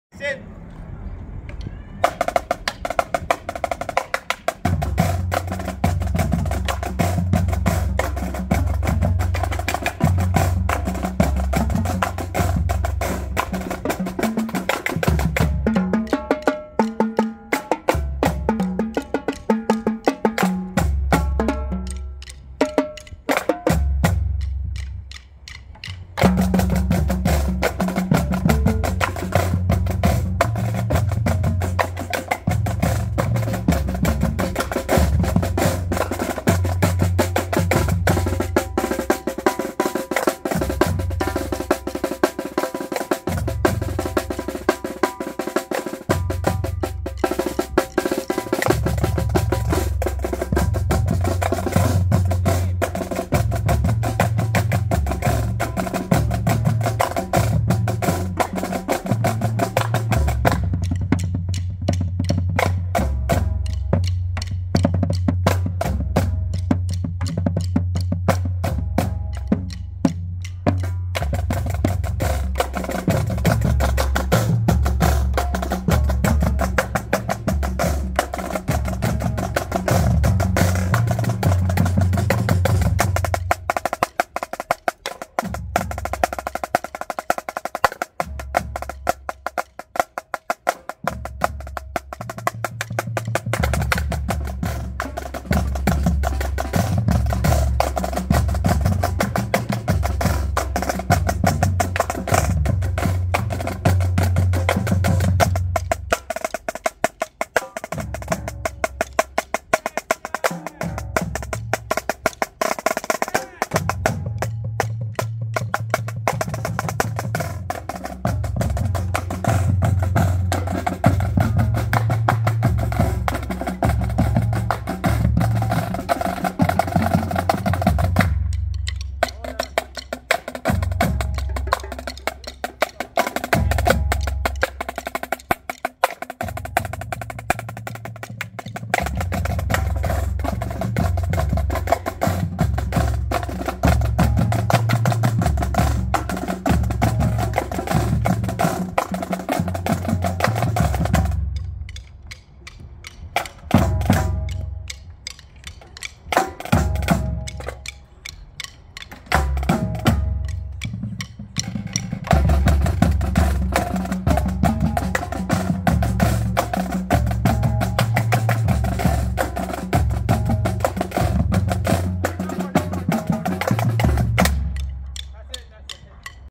Auditions at Soarin' Sound
For this video we tested our veterans by having them do solos! Make sure to drop a compliment! They tried hard and a brand new to it!